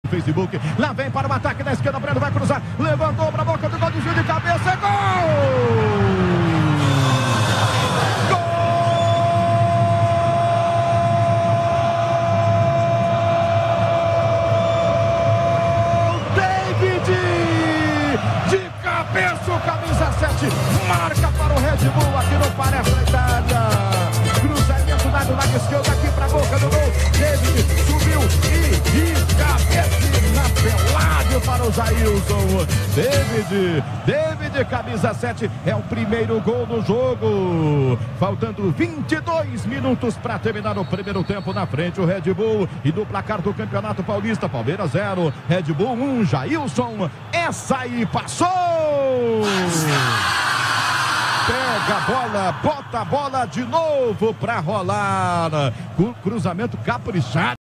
Gol de Deivid - Palmeiras 2 x 1 Red Bull Brasil - Narração de José Manoel de Barros
Deivid fez gol para o Red Bull no jogo contra o Palmeiras, pela 3ª rodada do Campeonato Paulista de 2018. Confira a narração de José Manoel de Barros, da Rádio Jovem Pan.